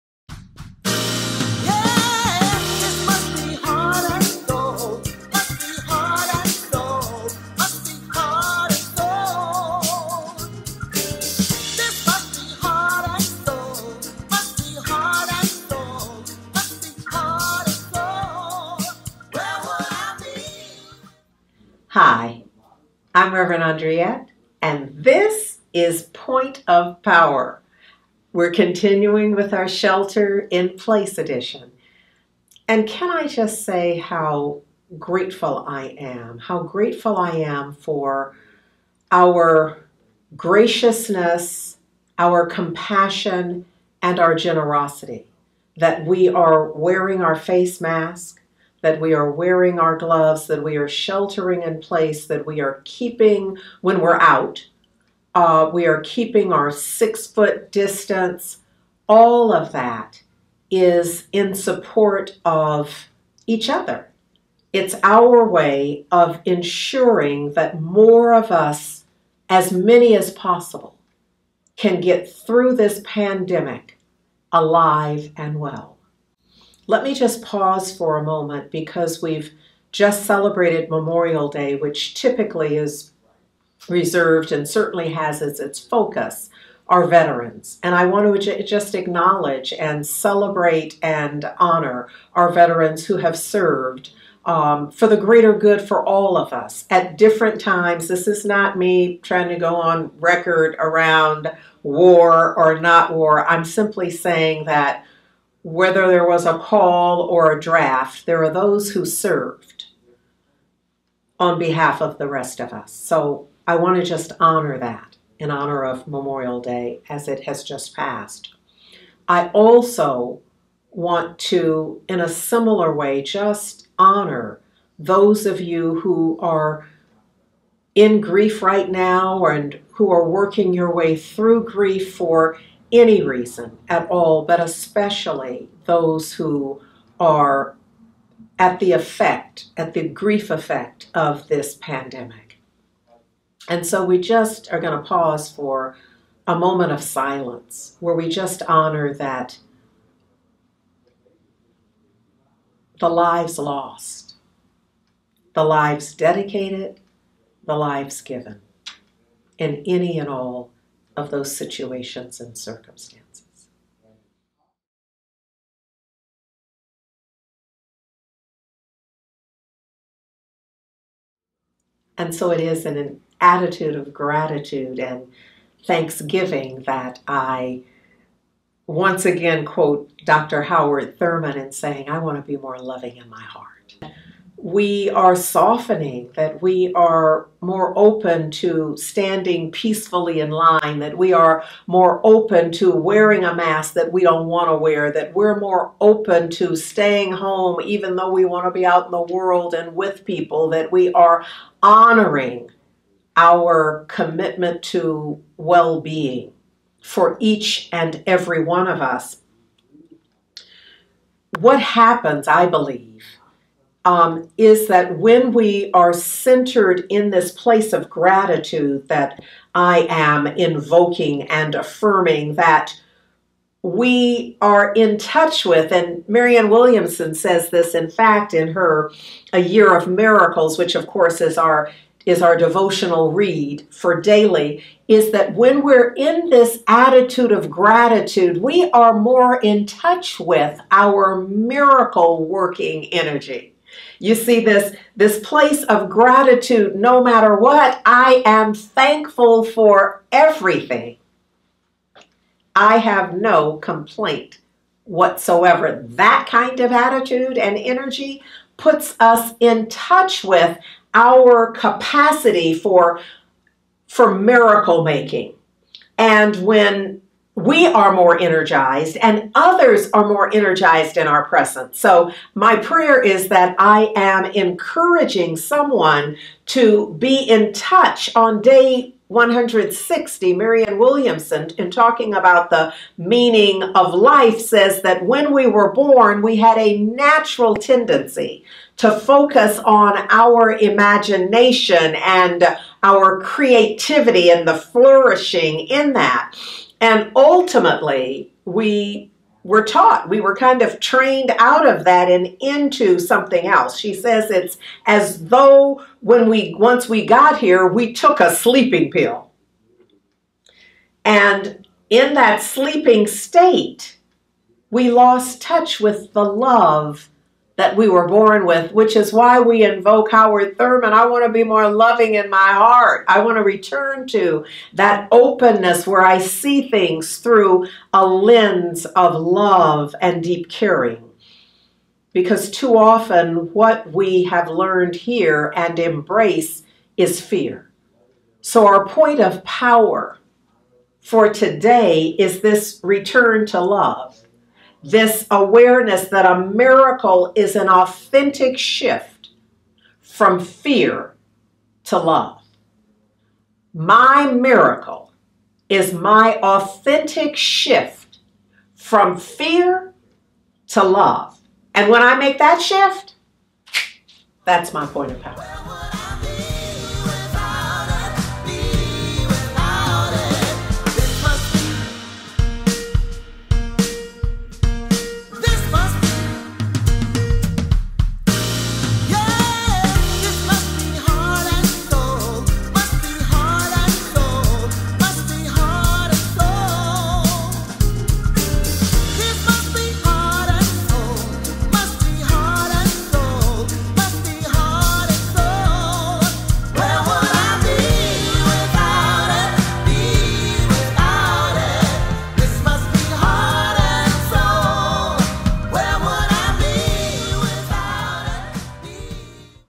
Miracle Working Energy
Citing Dr. Howard Thurman’s famous quotation, “I want to be more loving in my heart”, Rev. Andriette reminds us that when we see through a lens of love and deep caring combined with an attitude of gratitude, we produce miracle working energy. Even in the midst of a global pandemic where we find ourselves simultaneously torn by grief and stress, we can utilize miracle working energy. The energy itself lies in our ability to authentically shift from fear to love.

Join us every Sunday at 10:00a Pacific Time for Heart and Soul Center of Light’s online celebration service.